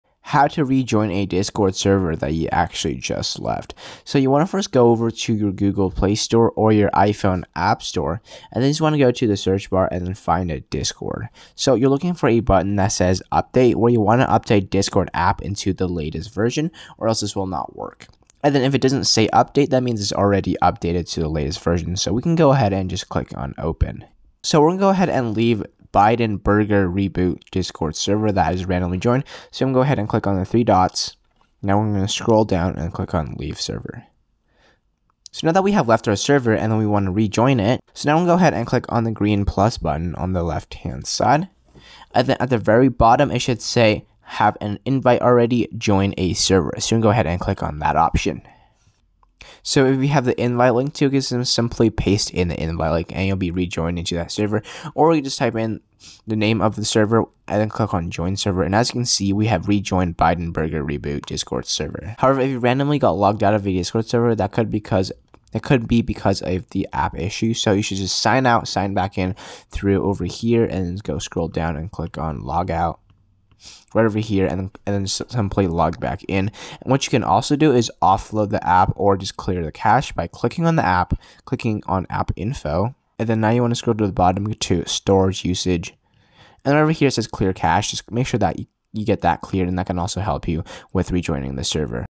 REJOIN A SERVER YOU LEFT ON DISCORD!
If you've ever left a server you were playing on Discord, fear not! In this video, we'll show you how to rejoin a server you've left on Discord.

This video is especially helpful if you've left a server in the middle of a game or if you just lost your connection. By the end of this video, you'll be able to rejoin a server you've left on Discord like a pro!